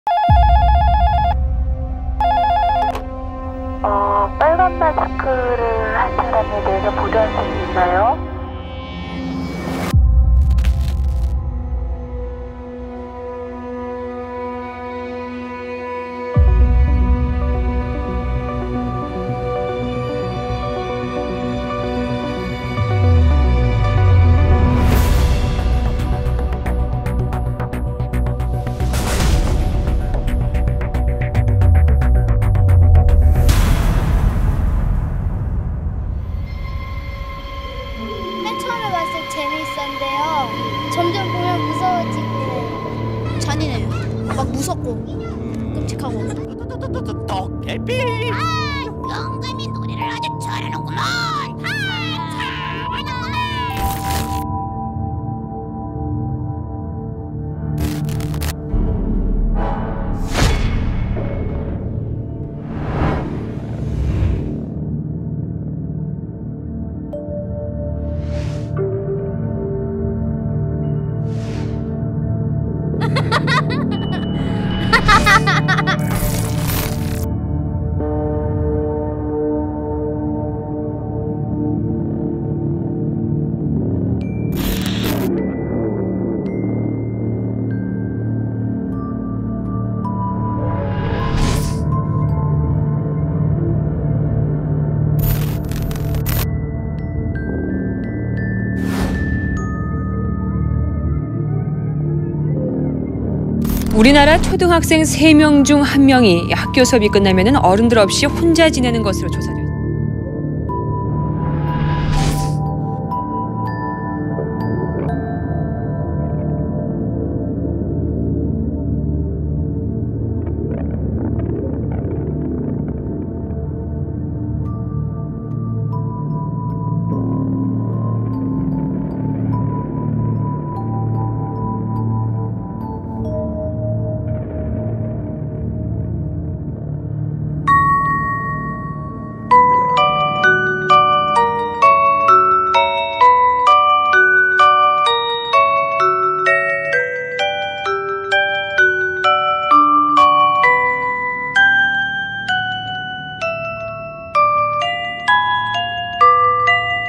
도시괴담 ‘빨간마스크’ 왜 아이들 사이에서 유행했을까? / 14F
공포의 도시괴담 '빨간 마스크'.
이토록 엽기적이고 잔인한 이야기가
왜 하필 어린아이들 사이에서 유행했을까요?